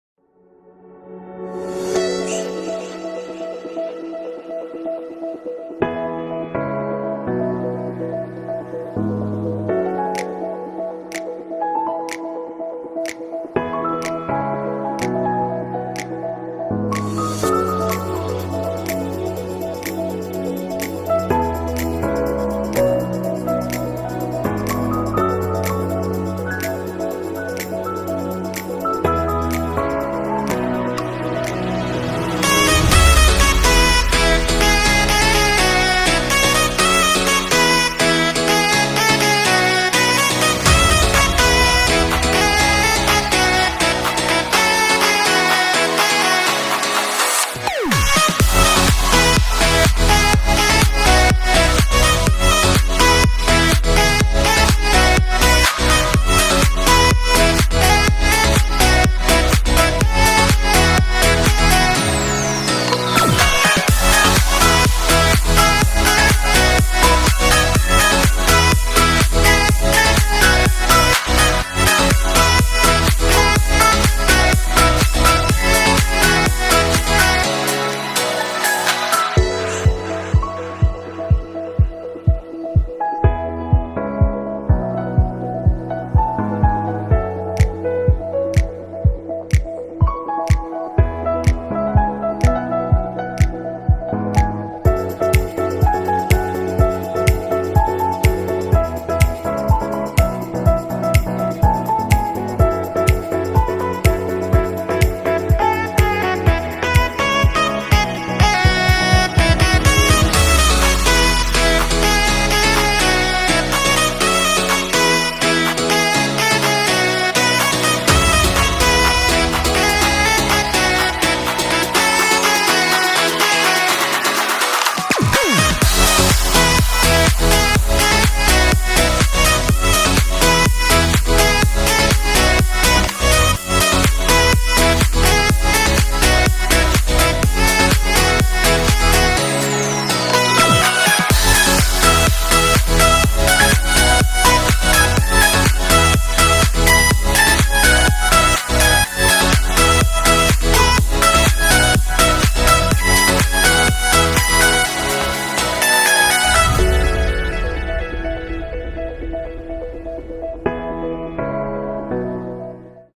第二屆粵來粵有趣-台灣賽區-綠色大地
Title :  Yilan Green EXPO 2019
Description : Experience at green expo
作品名稱 : 2019 綠色博覽會
作品註解 : 這一週免費來了2天 @@

Video : 5% Osmo Pocket
             95% GoPro Hero7